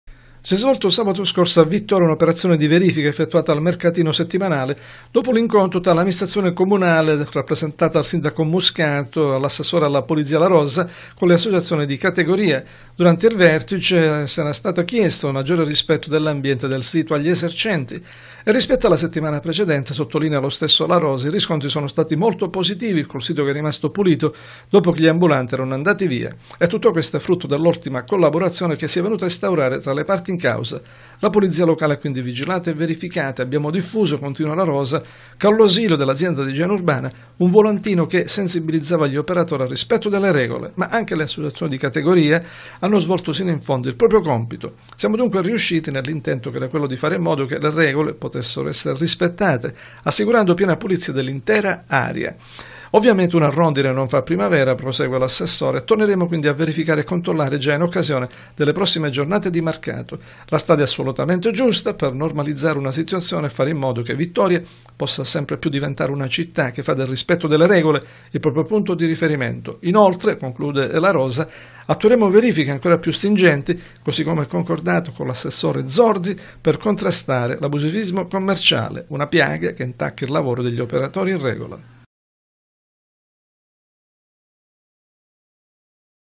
Mercatino sabato, riscontri positivi
Dopo le verifiche e i sopralluoghi dell'ultimo sabato al mercatino settimanale, i riscontri sono stati molto positivi lo sostiene l'assessore Andrea La Rosa che parla dell'ottima collaborazione instaurata tra le parti in causa.